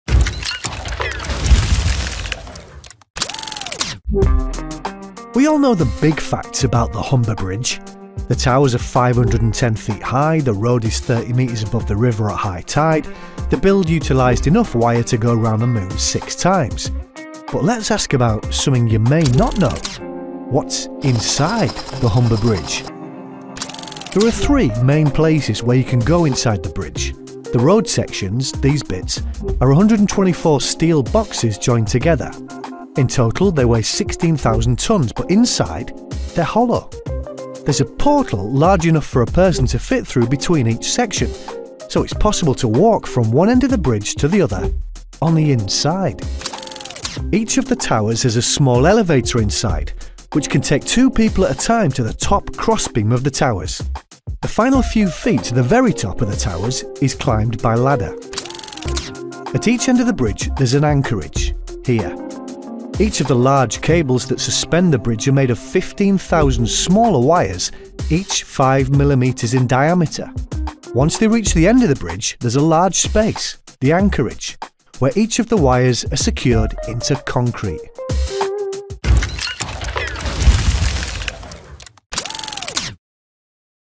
What's Inside The Humber Bridge?
Everyone in Hull knows the basic facts about the Humber Bridge - the length, the height etc - but not many people know what's on the inside of the bridge.

This film will tell you all about the three main places where the inside of the bridge can be accessed.

You can the find Supplemental Materials list for this film